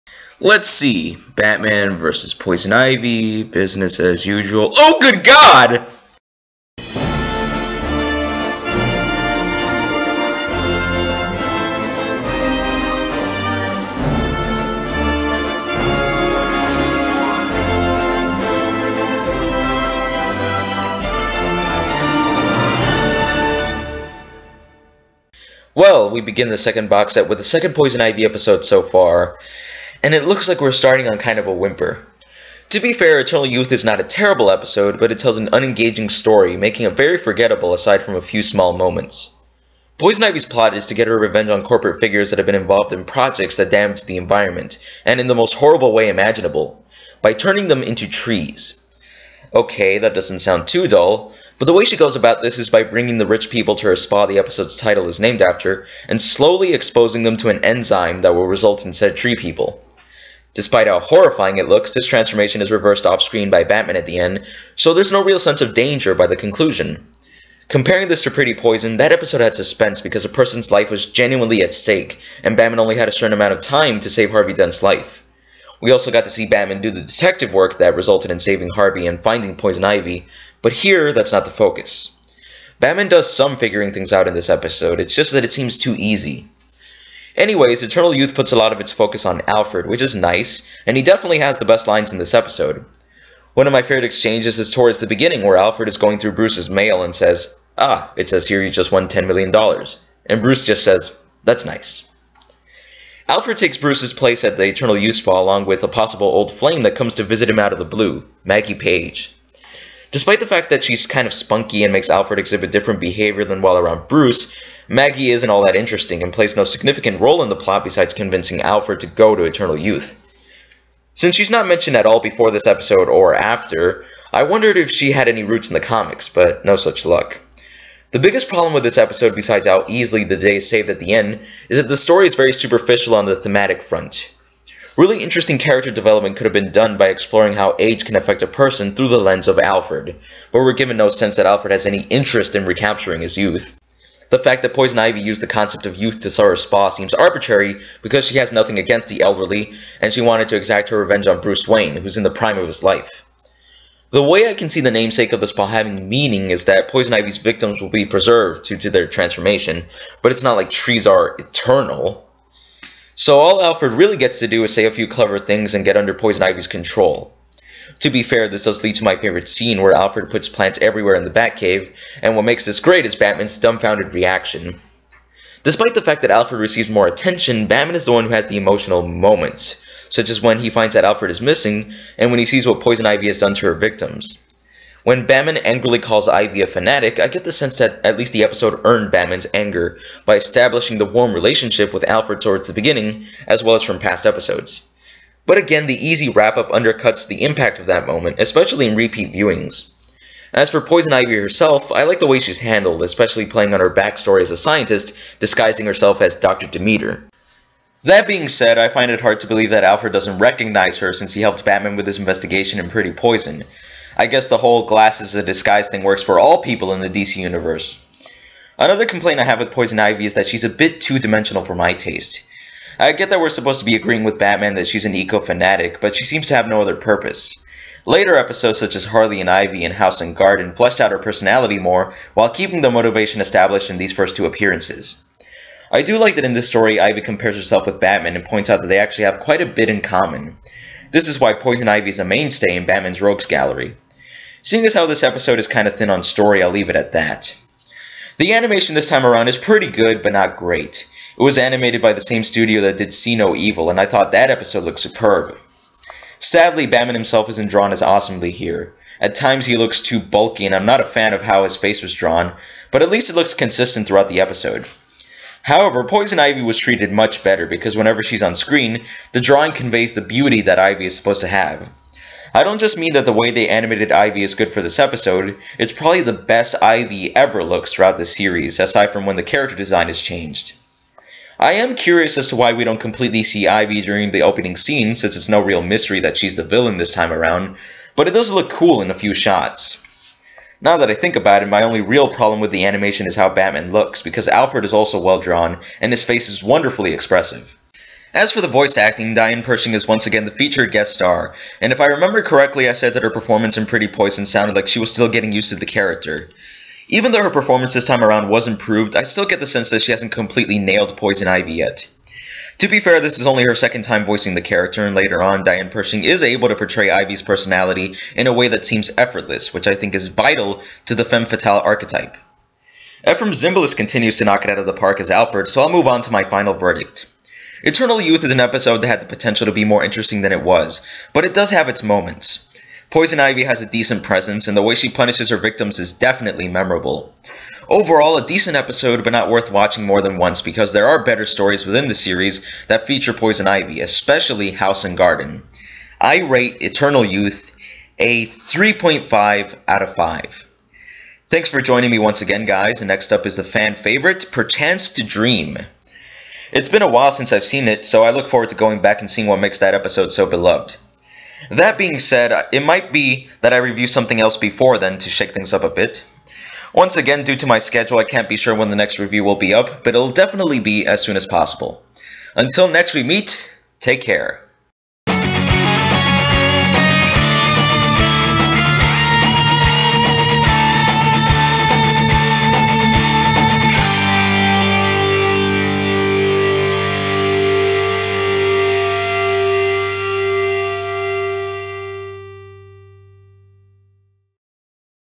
Batman TAS Review: Eternal Youth
Here's the next episode review with Poison Ivy! Sorry this one was brief especially because it's taking me long to put these out but I though it was best I be concise and precise this time lest I repeat myself or drag the review out.